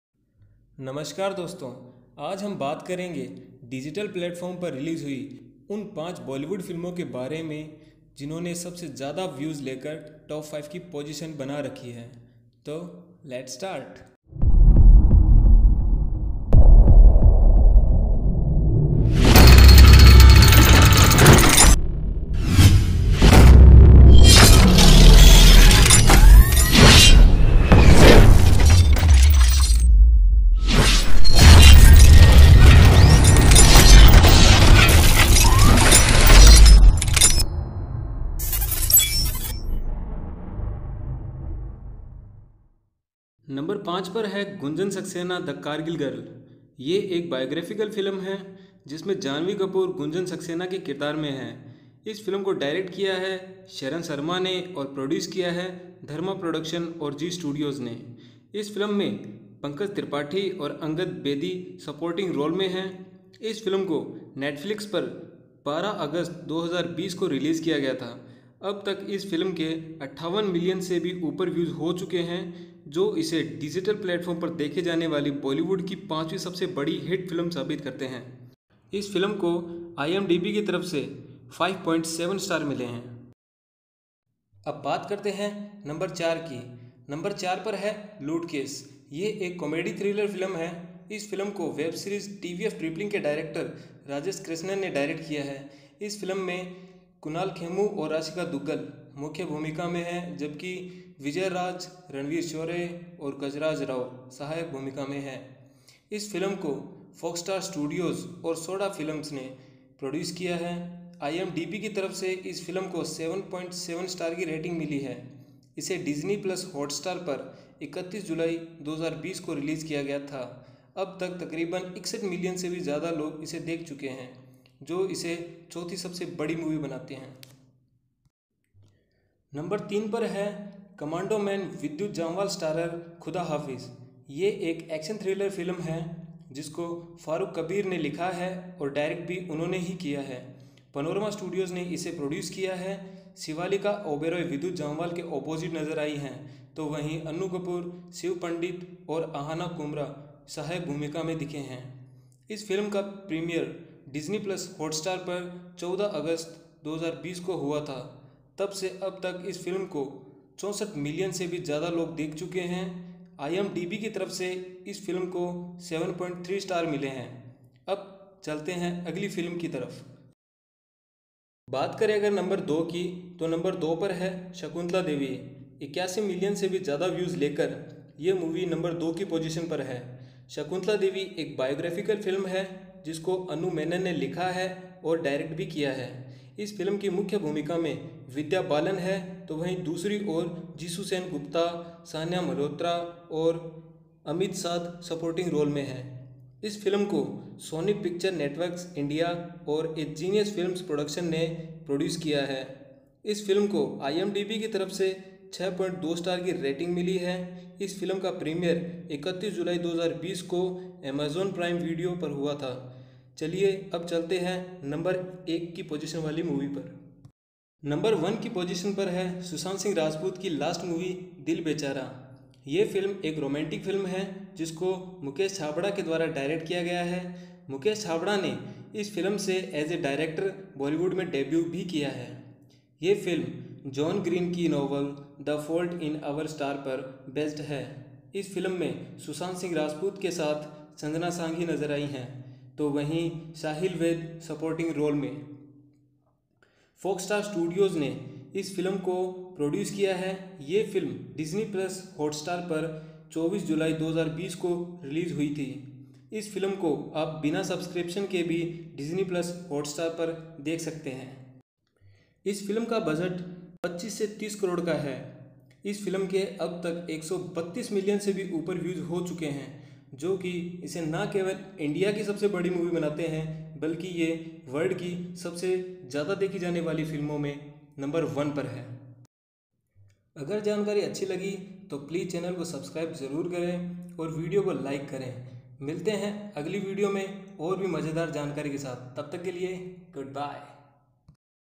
Top - 5 Most Watched Bollywood Movies On Digital Platform / OTT
Hello Friends

This is a informatic video on top 5 most viewed bollywood movies. All the data is accurate so if you are also a fond of movies then don't go anywhere.

Top - 5 Most viewed bollywood movies on OTT